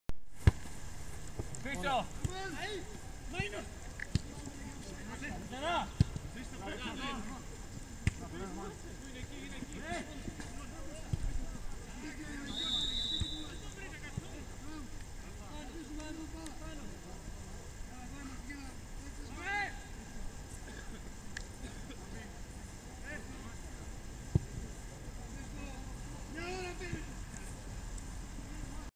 ΟΛΥΜΠΙΑΚΟΣ ΝΑΟΥΣΑΣ-ΑΚΑΔΗΜΙΑ ΒΕΡΟΙΑΣ 10-0 15/9/13
Ο πρώτος επίσημος αγώνας πρωταθλήματος για τον Ολυμπιακό Νάουσας απέναντι στην Ακαδημία Βέροιας.